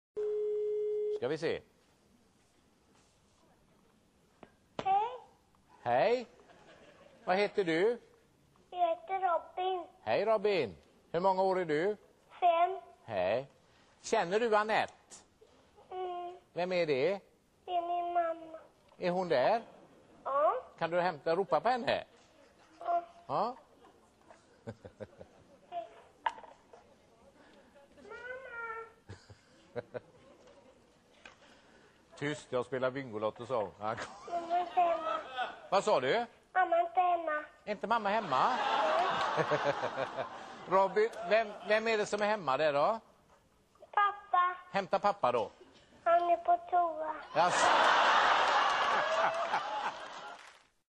Leif Loket - Ringer upp en 5åring
Leif Loket får problem. Mamma är inte hemma och pappa är på toa. Vem ska han nu dela ut miljonen till?